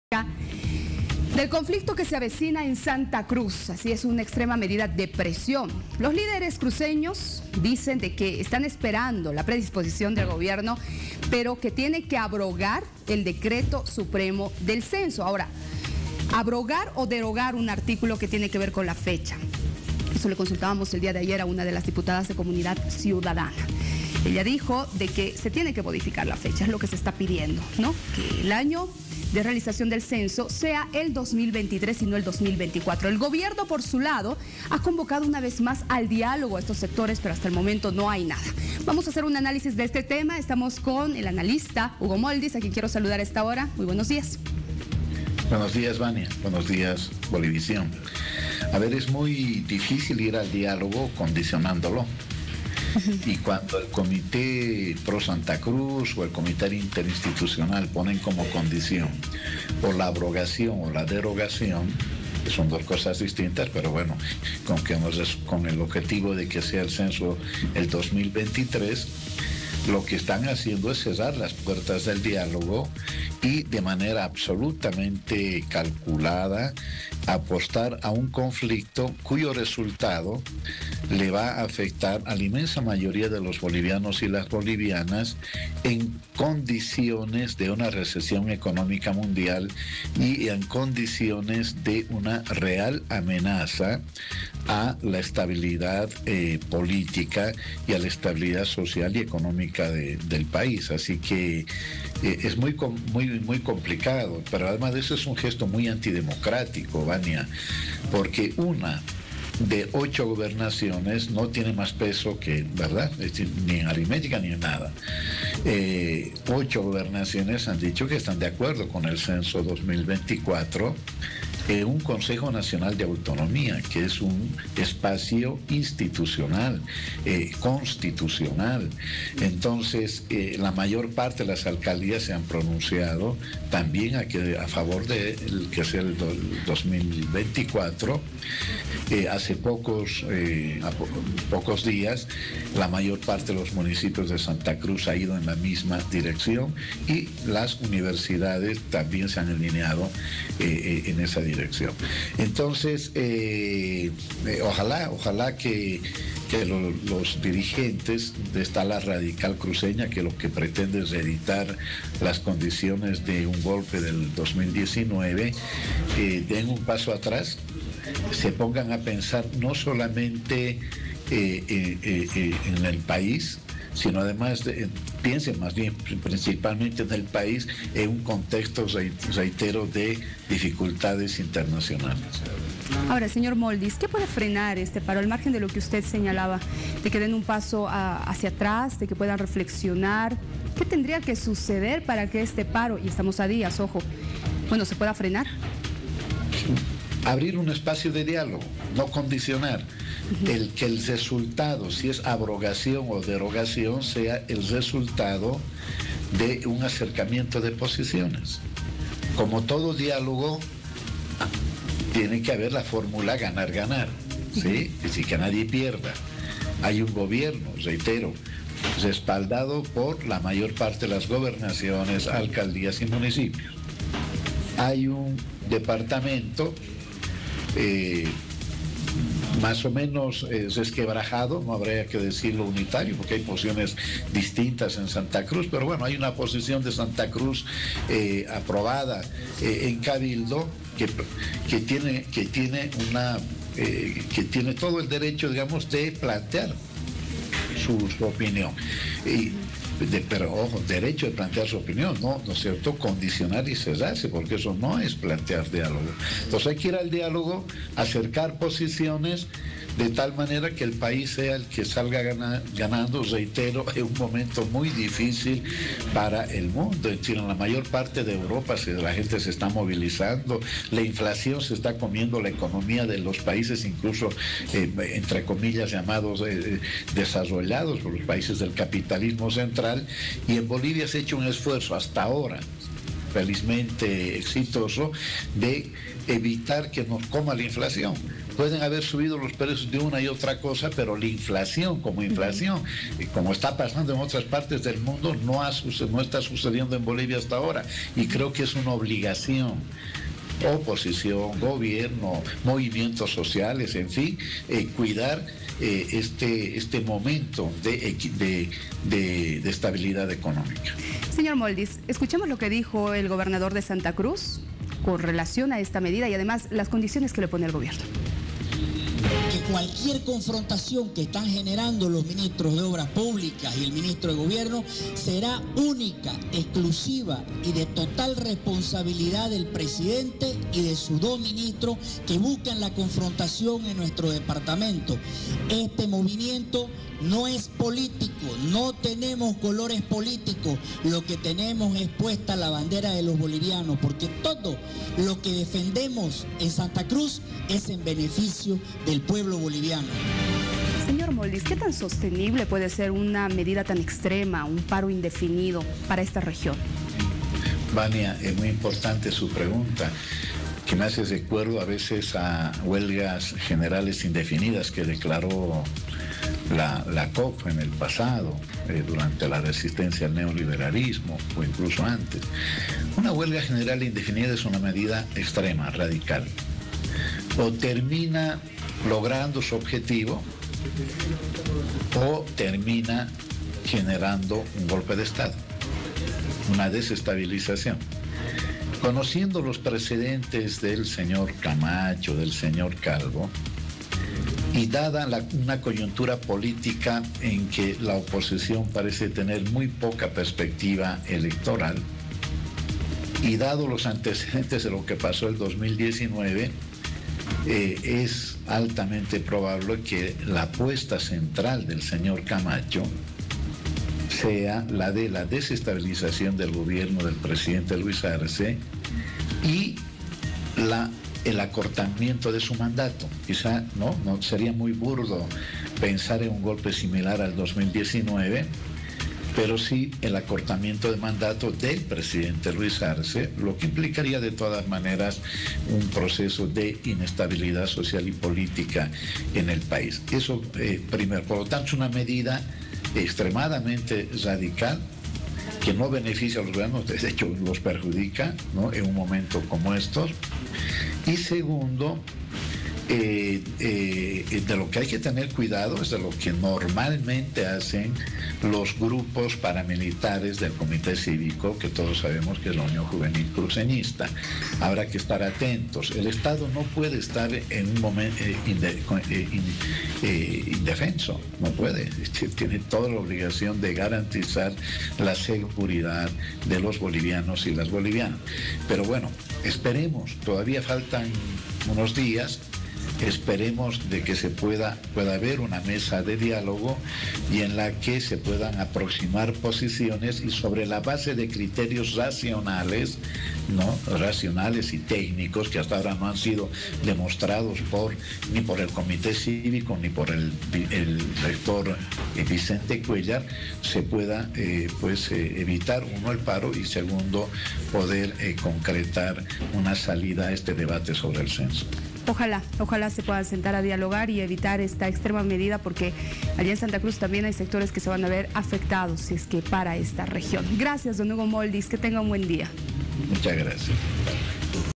Censo, tensión y paro ¿Qué puede suceder?
Se acerca el paro cívico en Santa Cruz.
Las autoridades de Santa Cruz continúan con su posición de cambiar la fecha para el próximo Censo, mientras que el Gobierno ha planteado un nuevo diálogo con esta autoridades, pero hasta el momento no se han pronunciado acerca de esta invitación.